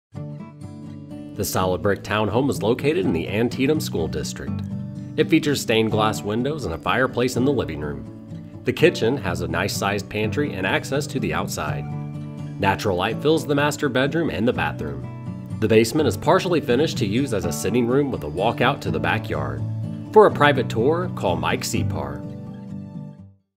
115 N 25th Street Reading, PA | ColdwellBankerHomes.com
115 N 25th Street, Reading, PA

This solid brick townhome is located in Antietam School District. It features stained glass windows and a fireplace in the living room. The separate dining room includes a closet. The kitchen has brand new countertops, a ceiling fan, laminate flooring, a nice sized pantry and access to the outside.